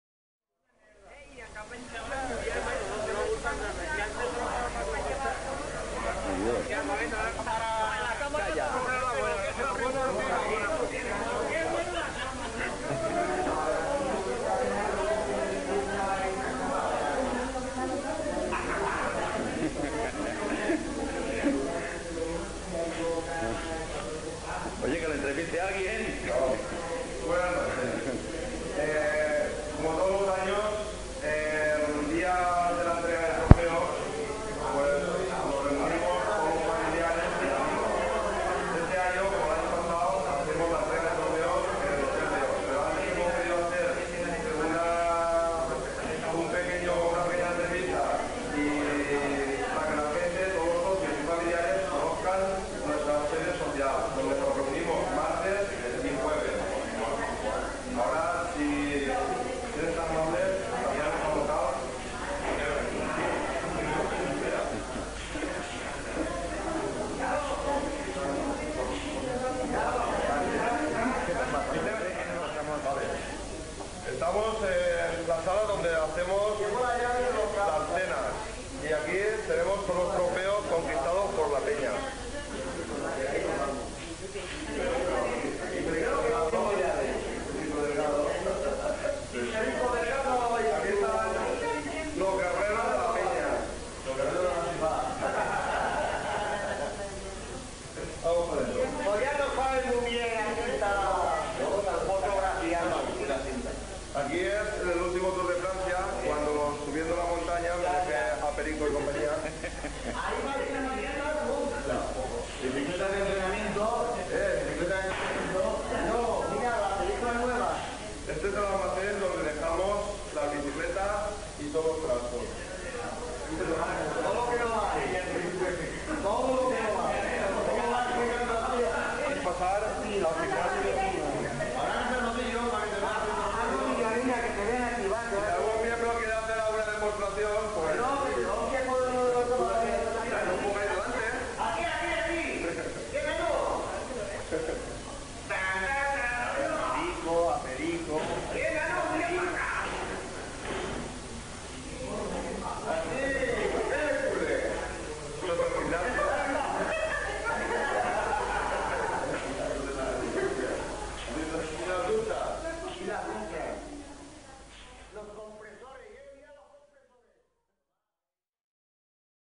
Antiga seu de la Penya Ciclista Excursionista de València
En l'antiga web de la Penya Ciclista Excursionista de València vaig publicar diversos vídeos. Per a celebrar el 90 aniversari de la Penya pujaré a aquest canal alguns d'ells que he trobat a l'ordinador.